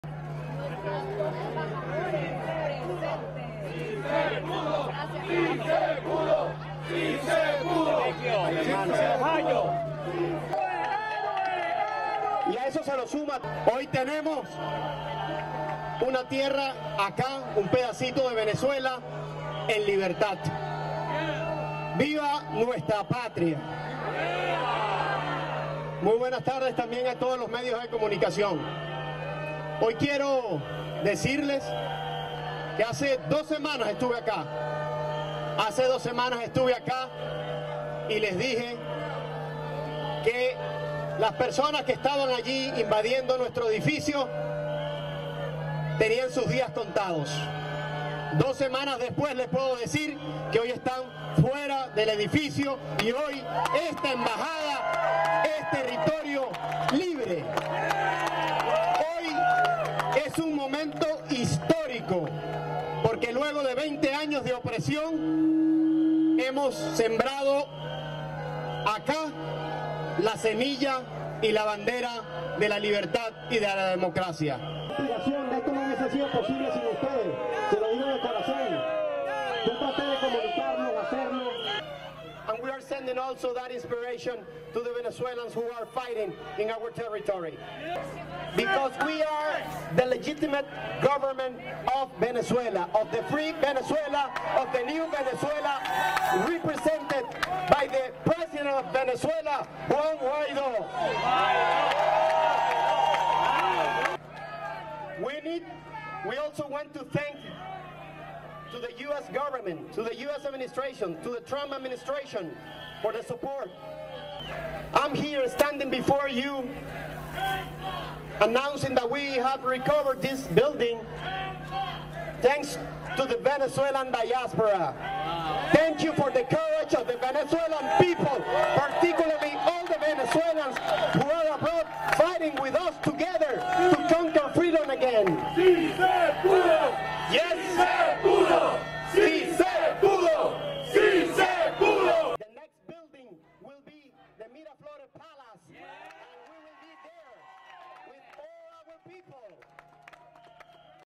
Guaido's US envoy hails Venezuela embassy clearing
(17 May 2019) Venezuelan opposition's envoy to the United States, Carlos Vecchio, announced the recovery of the Venezuelan Embassy  building on Thursday, following the arrests of four demonstrators who were staging a protest inside the building in Washington DC.
Vecchio addressed the crowds across the street from the embassy.
"Today is a historic moment because after 20 years of oppression we have planted here, the seeds and the flag of freedom and democracy", he said.
The protesters consider Nicolas Maduro to be the legitimate Venezuelan president.
But the United States and more than 50 other countries say Maduro's recent reelection was fraudulent and are backing congressional leader Juan Guaido's claim to the presidency.
Guaido's newly named ambassador had requested the help of US authorities in clearing the building.
The protest started more than a month ago with at least 30 activists staying at the embassy, but their numbers gradually dwindled.